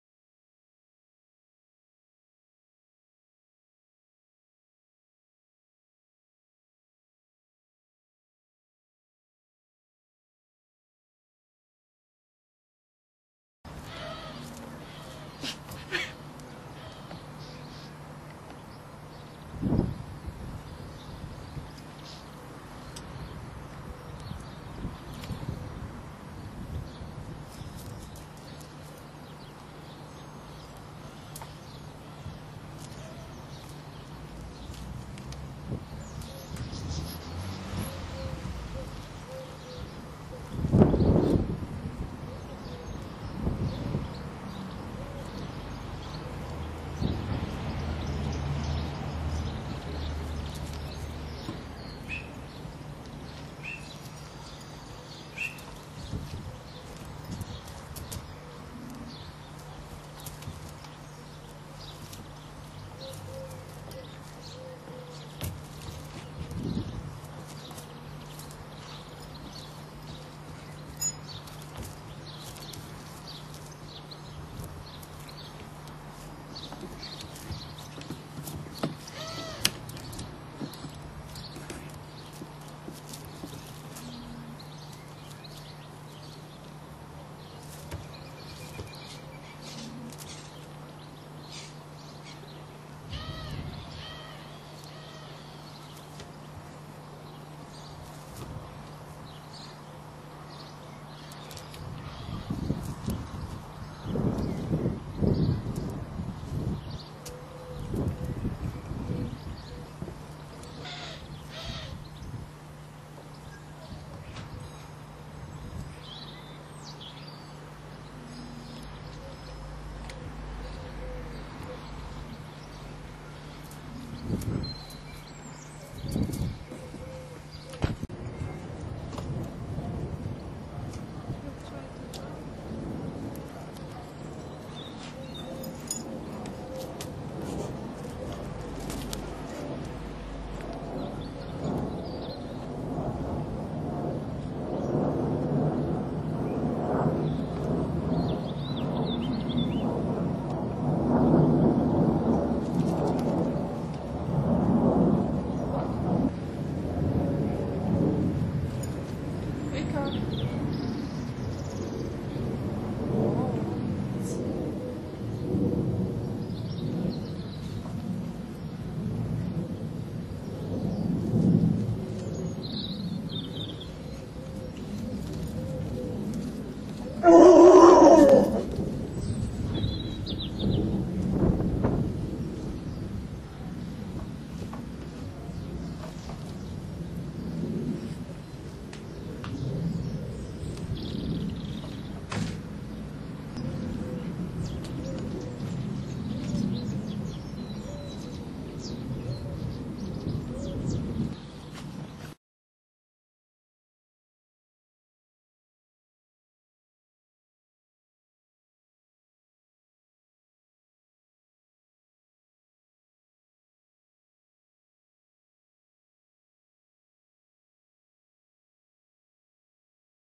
Our Mad Squirrel
This squirrel comes around to see us every day. It puts its life at risk every time it comes to see us. My Wolf/Dog Lika would love to get a hold of it, but it seems to be pretty savvy about getting out of his way. It stole some of his soft toys this morning and then took them on top of the roof of some cages in my back yard. He then proceeded to kill them all. It was really funny stuff, so I thought I would share it with the rest of the World.

Regards

Roy